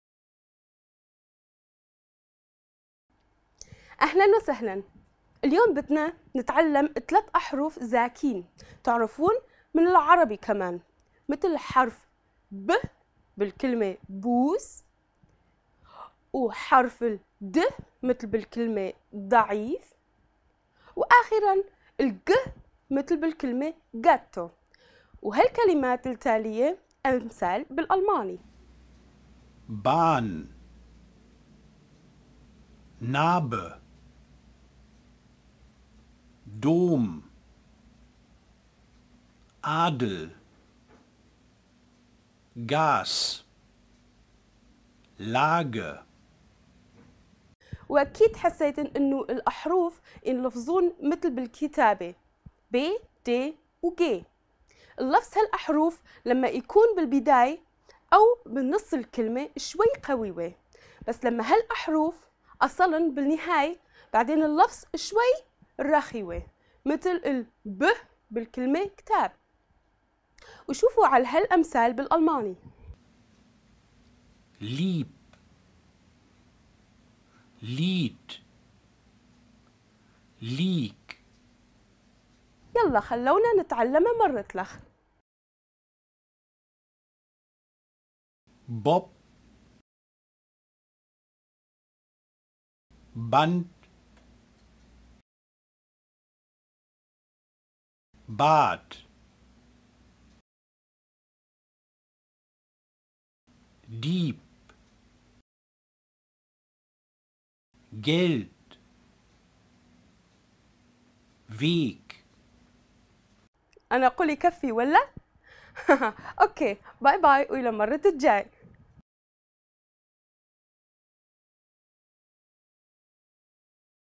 DEU4ARAB_203 - German / b, d, g / for Arabic Learners of German
This short video provides several examples for Arabic learners of German using the so-called "voiced plosive" consonants / b, d, g /. They all exist in Arabic, however, in German they are devoiced in final position. Hence, this video is basically an exemplifcation of the differences between initial and final "b", "d", and "g" in German.